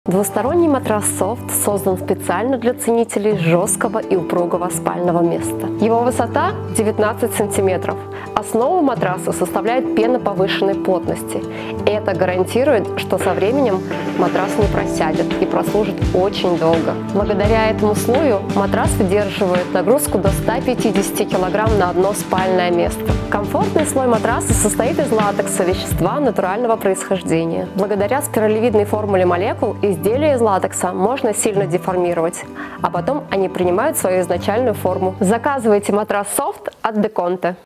Экспертное мнение Матрас Беспружинный Софт
Матрас Софт включает в себя слой поролона повышенной плотности (80 кг/м3) - 140мм. Это гарантирует, что матрас будет держать свою форму долгие годы и не просядет. В матрасе также есть слой латекса - 40мм, он придает упругость спальному месту и снижает обратное давление, обеспечивает нормальное кровообращение, что позволяет комфортно спать. Беспружинный матрас Софт является двусторонним, с одной стороны матрас жесткий, подходит людям с проблемами позвоночника, с другой стороны жесткость матраса - средняя, считается универсальной и подходит большинству людей. Верхний комфортный слой гипоаллергенный трикотаж, стеганный на синтепоне, обеспечивает приятные тактильные ощущения.